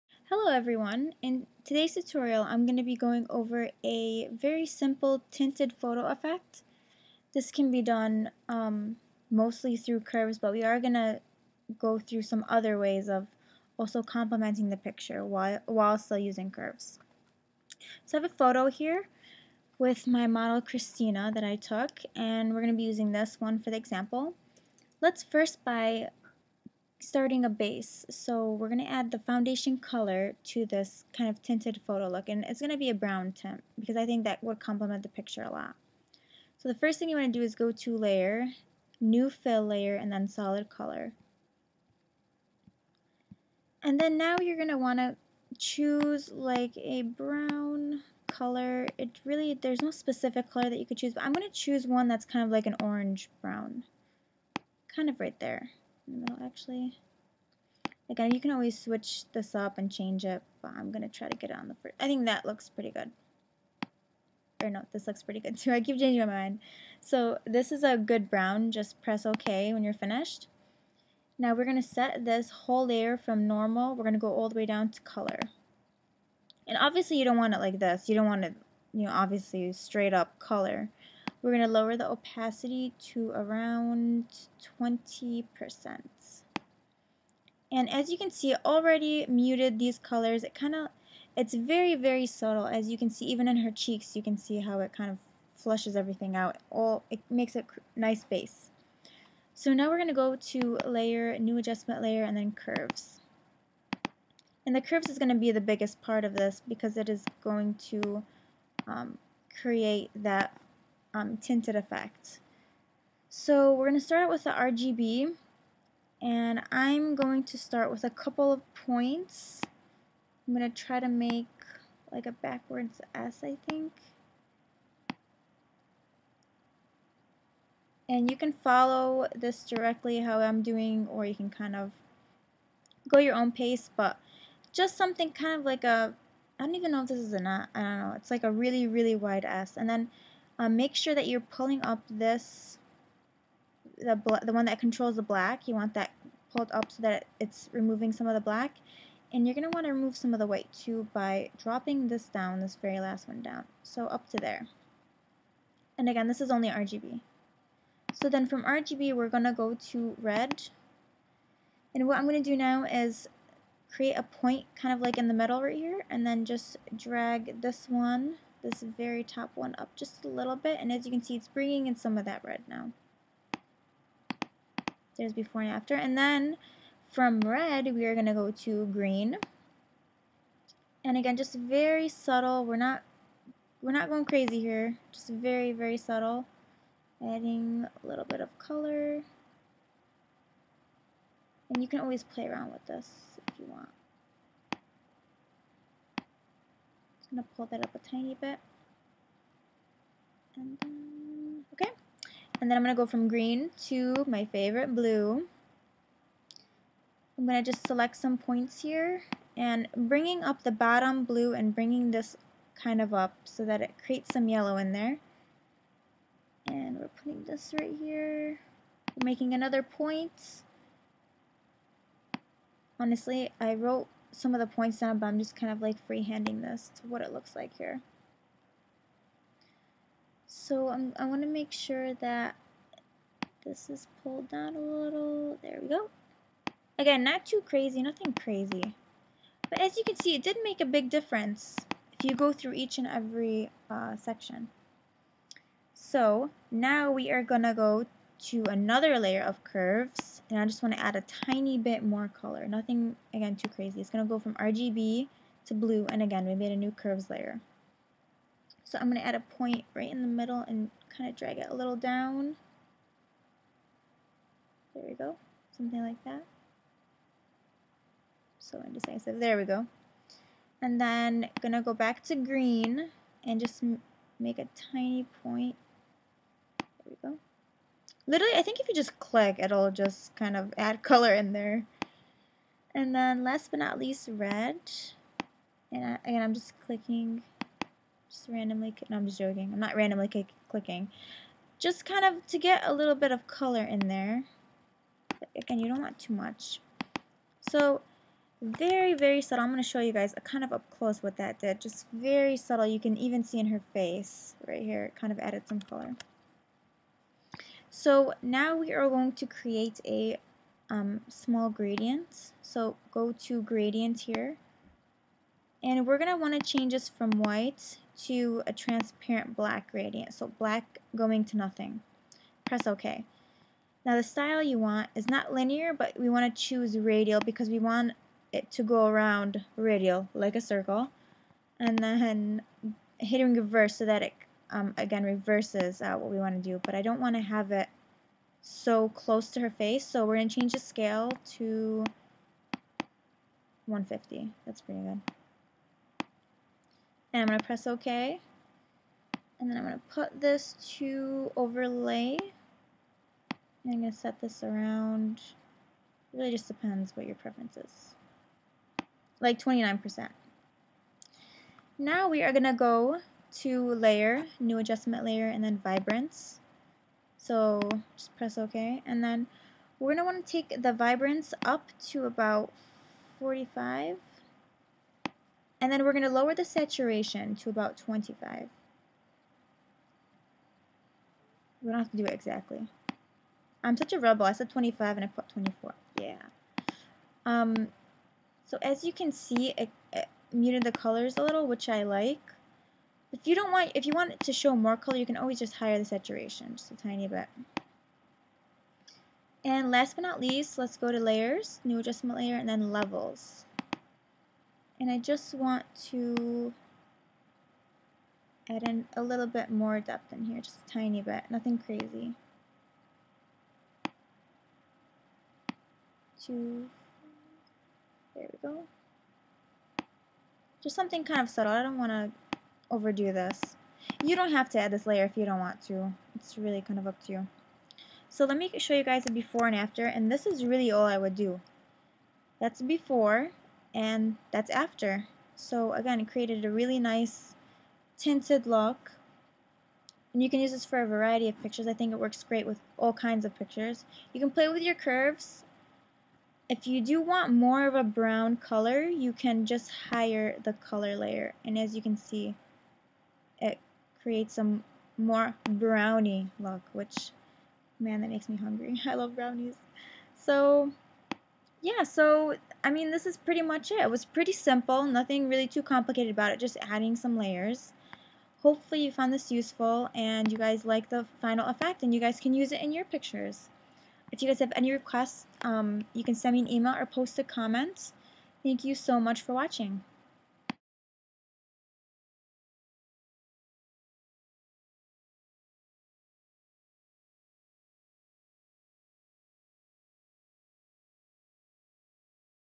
Soft Vintage Photo Effect - photoshop tutorial
Super easy soft vintage effect. Great for fashion photos!